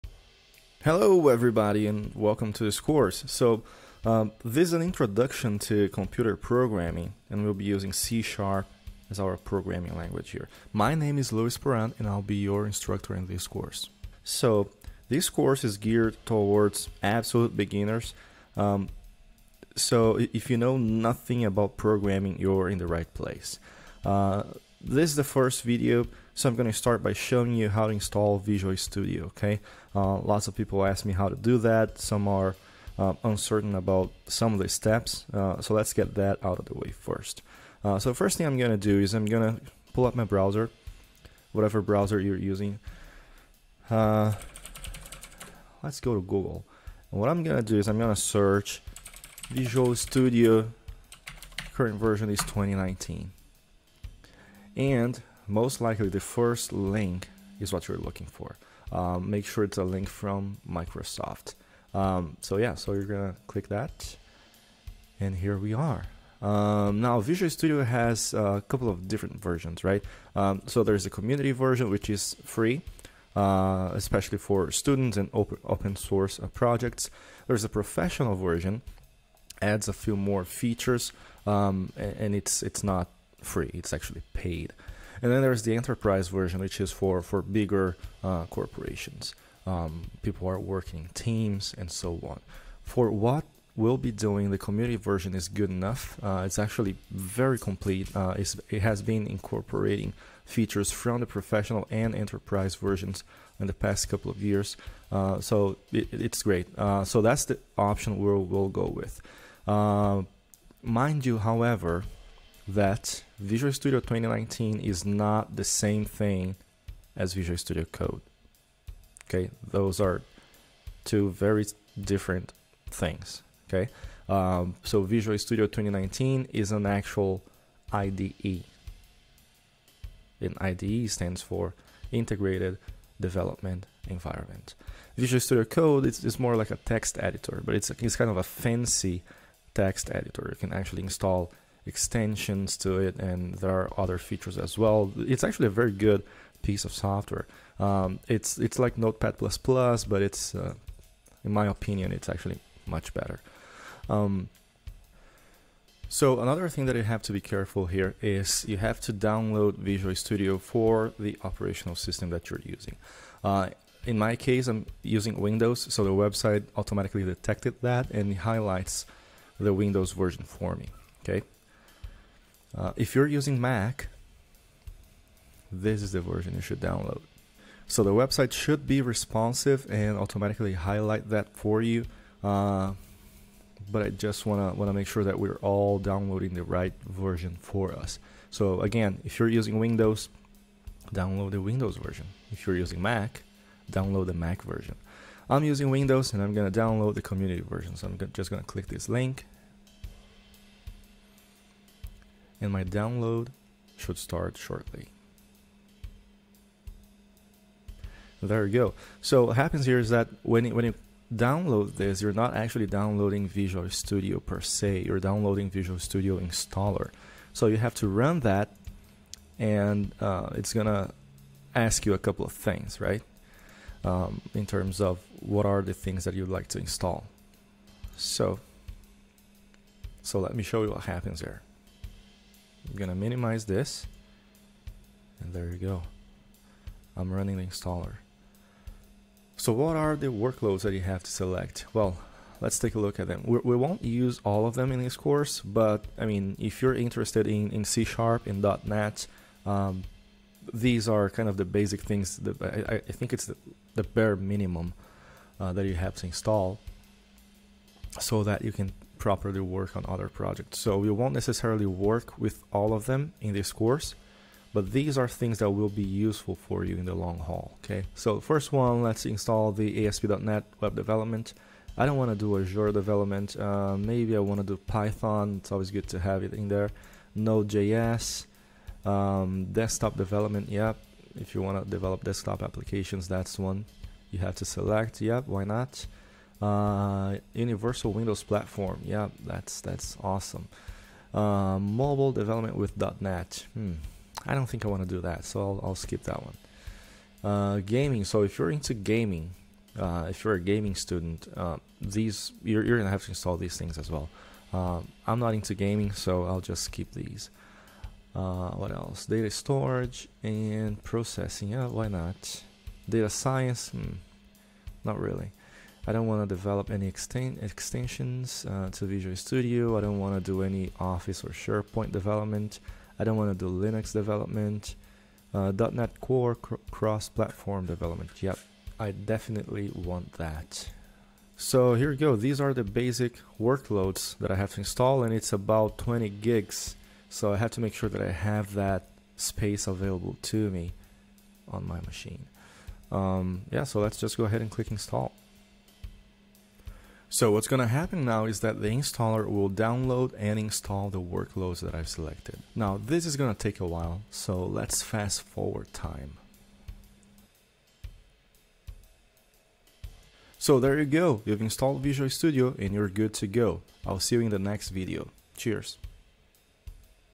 How To Install Visual Studio
In this video you will learn how to install Visual Studio 2019.